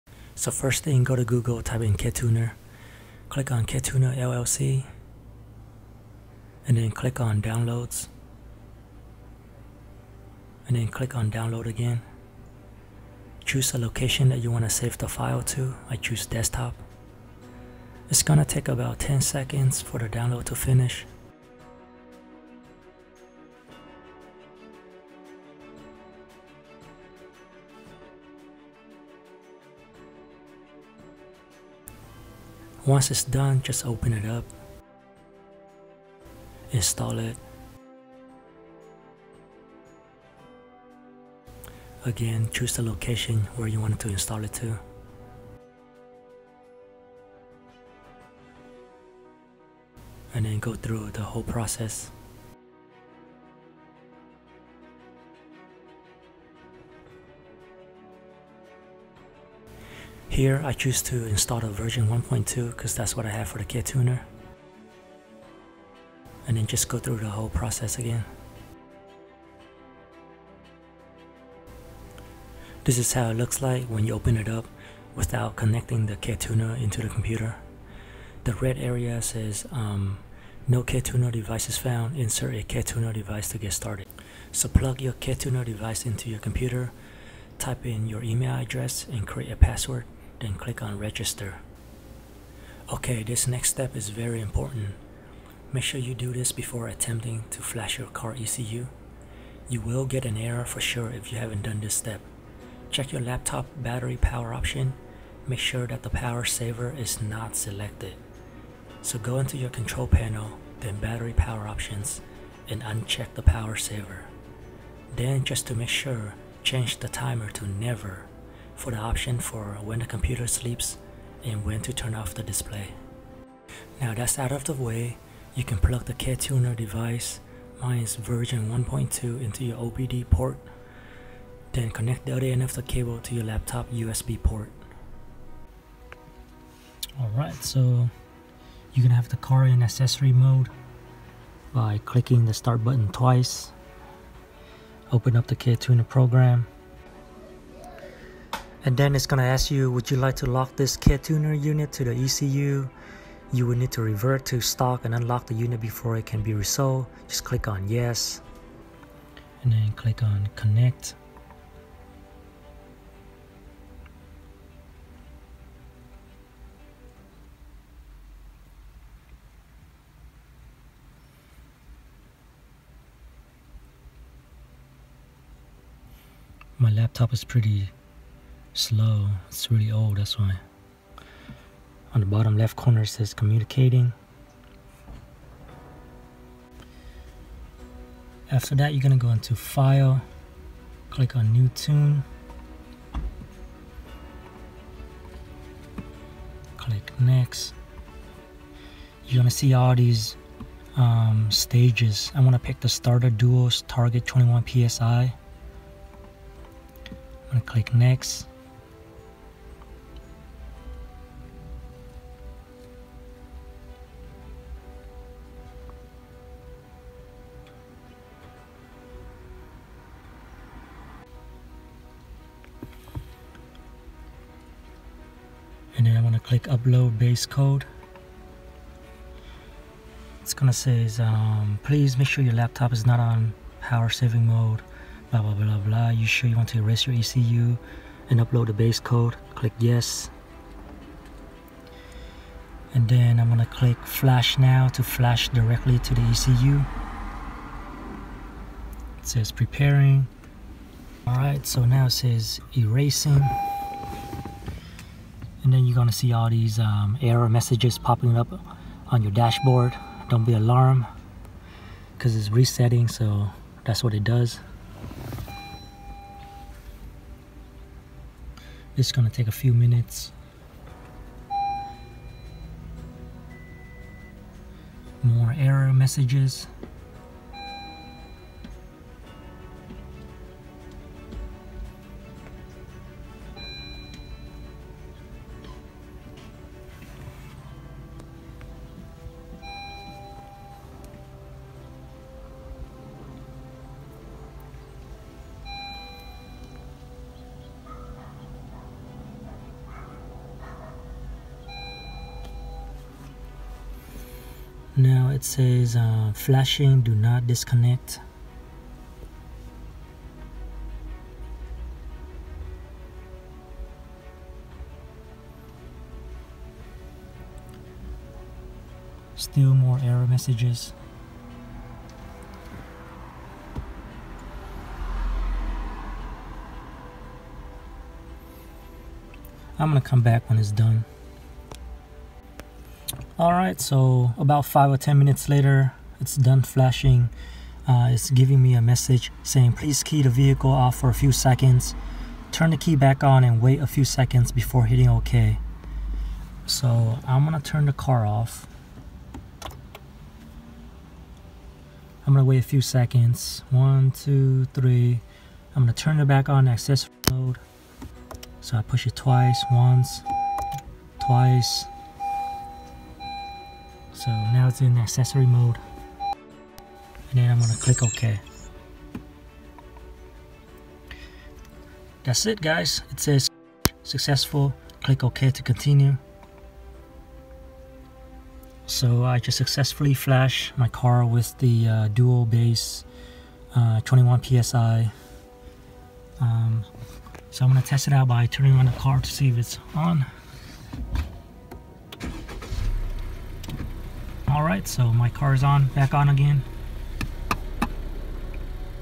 How to install & flash Ktuner
This is how I install the Ktuner program, then flash my honda civic hatchback 10th gen with it.